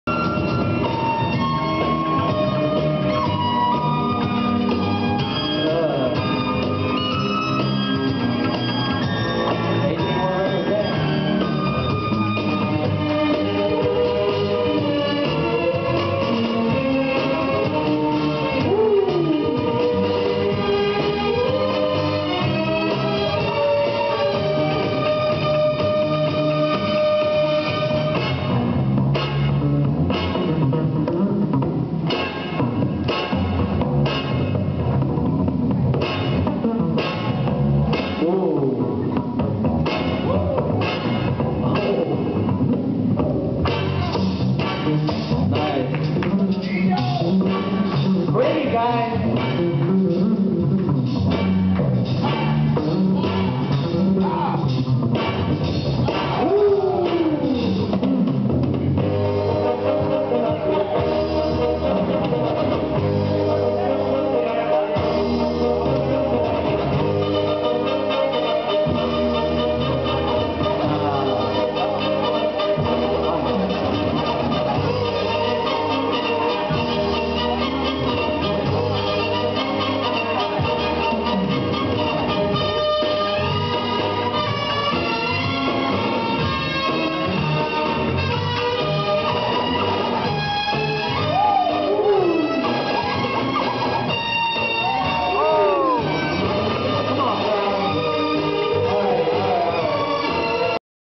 Service - Da Father vs. Barakuda
Da Father (SSC: Street Style Crew) vs. Barakuda (Anthrax Crew/ Team Anthrax?) at Salinas "Service" Jam.